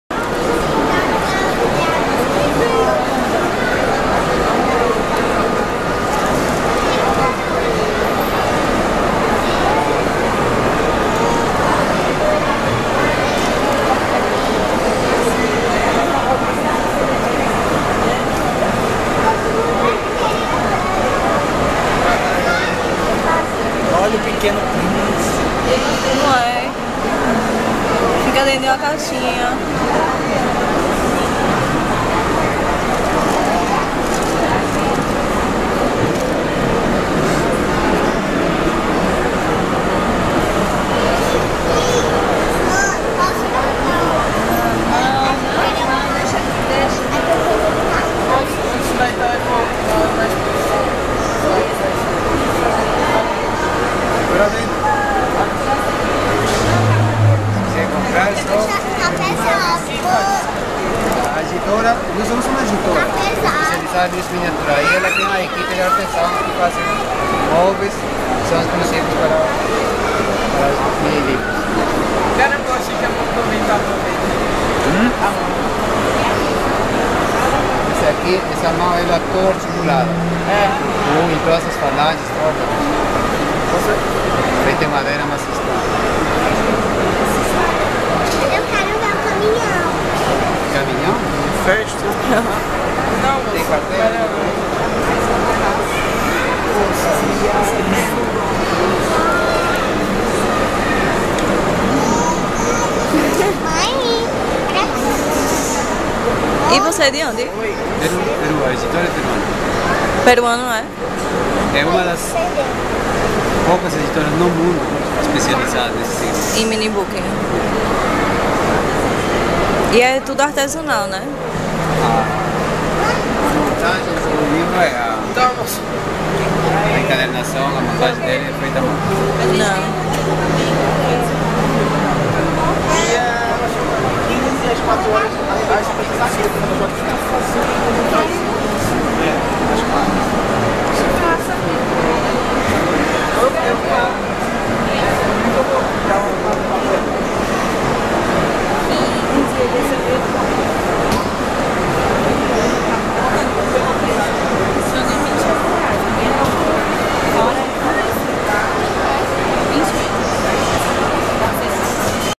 Minibooks feitos à mão no Peru
Uma editora peruana que faz minibooks na América Latina.
Além dos objetos para guardar os livrinhos, tem cadernos artesanais também.